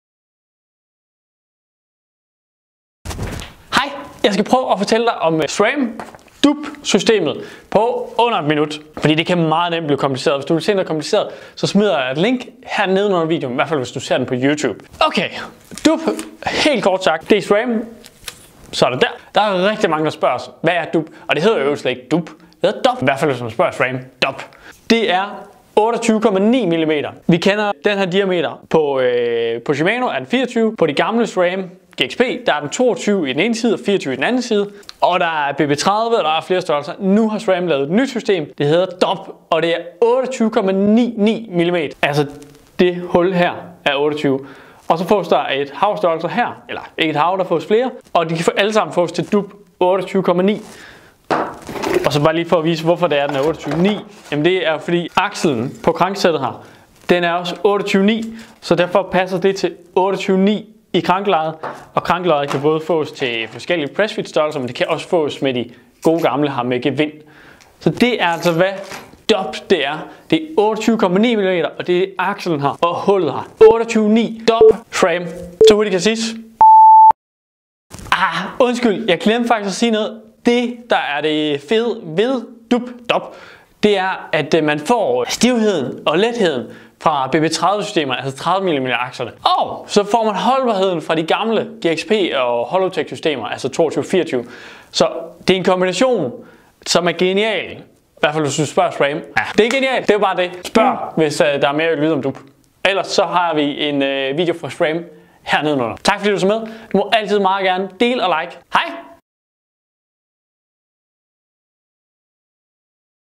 Hvad er SRAM DUB? Forklaring på 1 minut!
Erik fra Cykelgear forklarer her hurtigt hvad SRAM DUB går ud på, så du ved hvad det handler om.